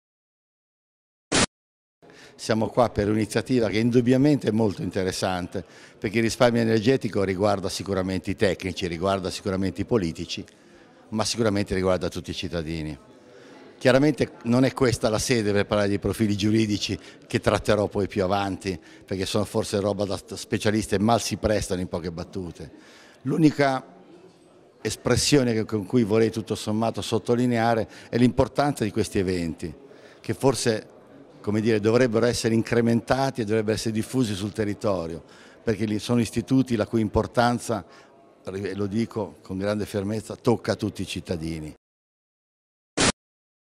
Umberto Realfonzo, Consigliere di Stato
Il Consigliere di Stato ha approfondito, nella quarta giornata della Settimana 2018, le procedure di affidamento del contratto pubblico di rendimento energetico.